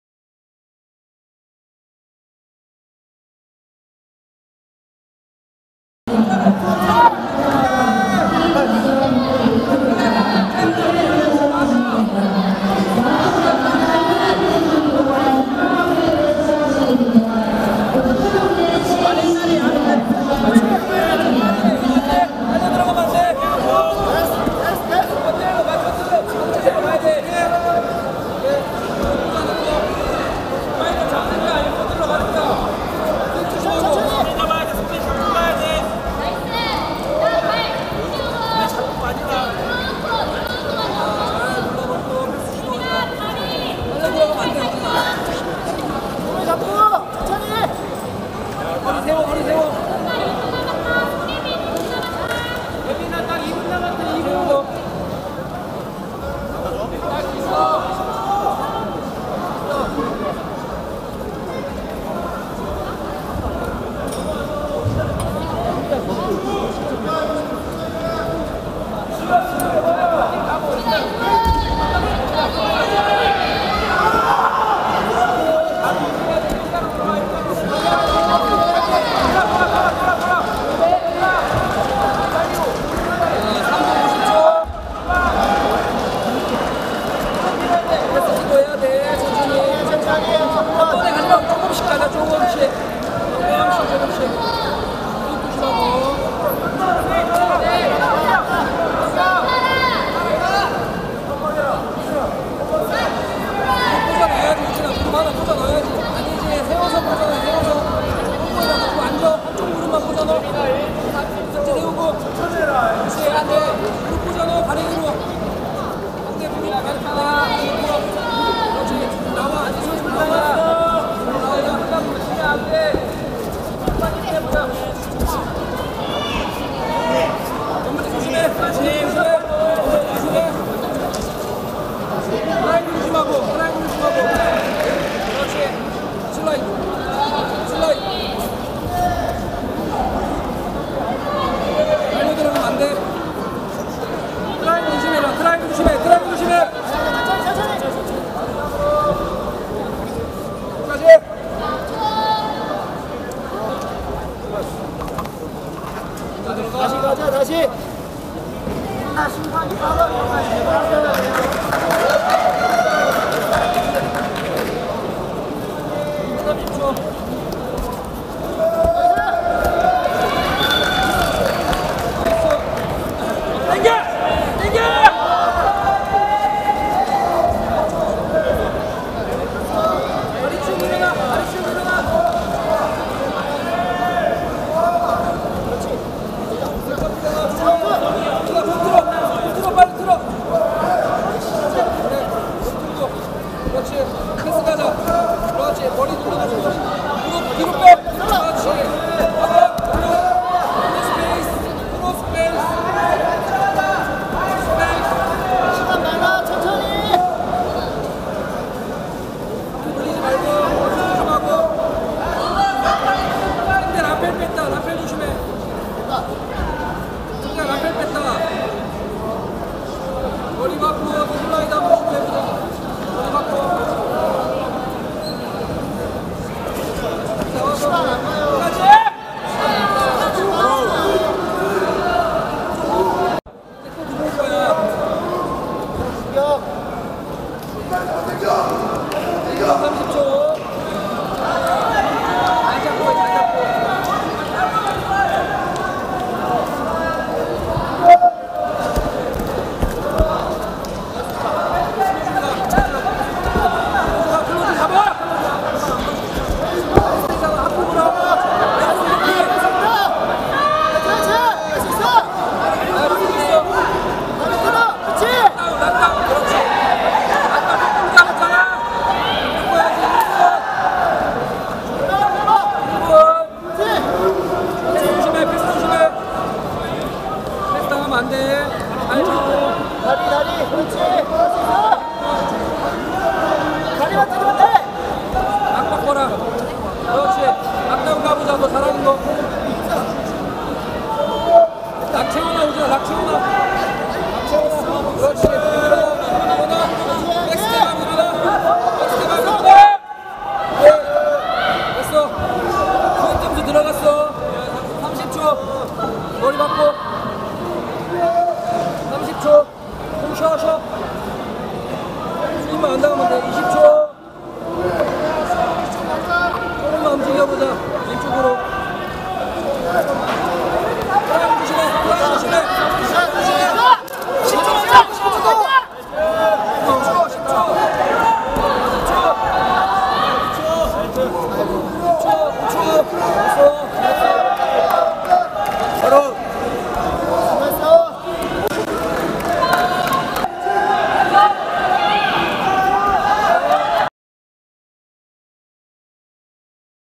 [리얼주짓수] 10매트 26경기
[리얼주짓수]
The 3nd Real JiuJitSu DaeJeon Open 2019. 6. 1 대전충무체육관
해당 매트는 당일 장비문제로 인하여 부,소속,체급,선수명을 확인할 수 없어 편집이 불가하여 본영상대로 게시하였습니다. 본 영상속 선수들께선 신청부ex)초등부,중등부,성인부 및 소속,체급,선수명을 댓글로 남겨주시면 두 선수 모두 확인시 수정하도록 하겠습니다. 불편드려 죄송합니다.

※ 신청부ex)초등부,중등부,성인부 및 소속,체급,선수명이 모두 확인시 수정 가능합니다.